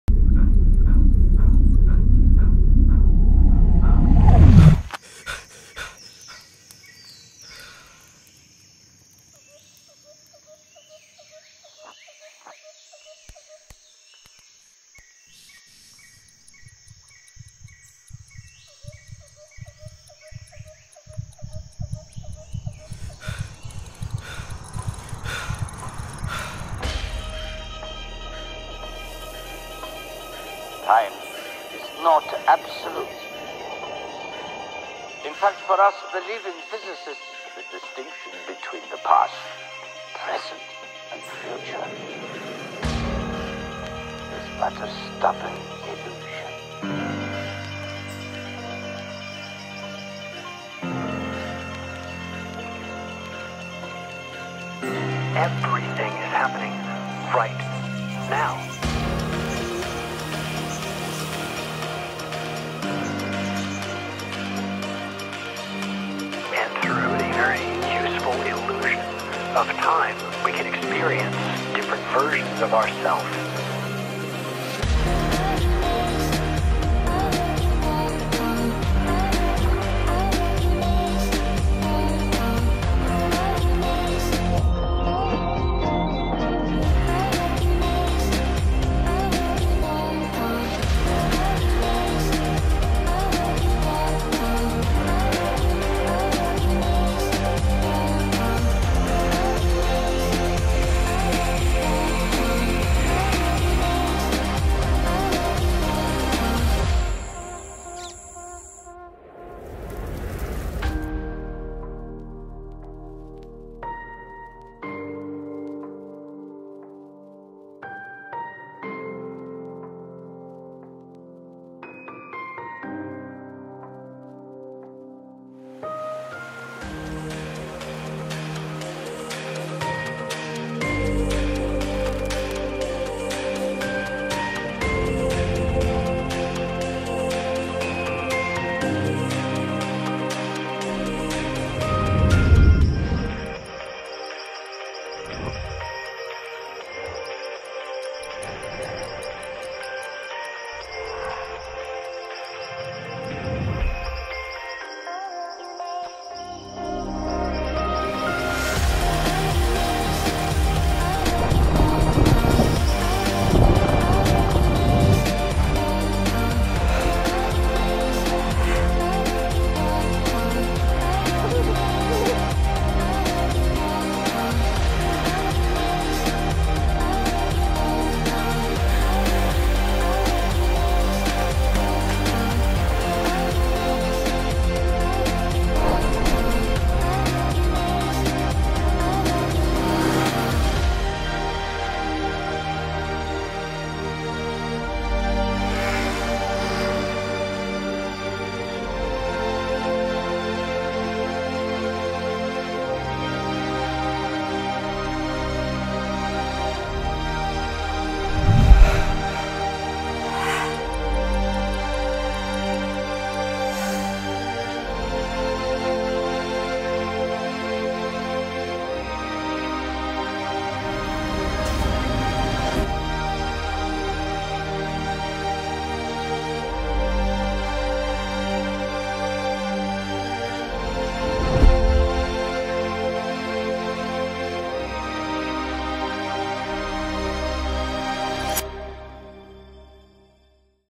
Laos - Cambodia | An Illusion Of Time | Cinematic Travel Video
Laos has always been on my list to travel to. This was the journey of a lifetime! Meeting so many new loverly people on the way, learning about the different cultures and traditions, visiting amazing places and views. Laos made us feel like we were lost in time.

A journey we will never forget!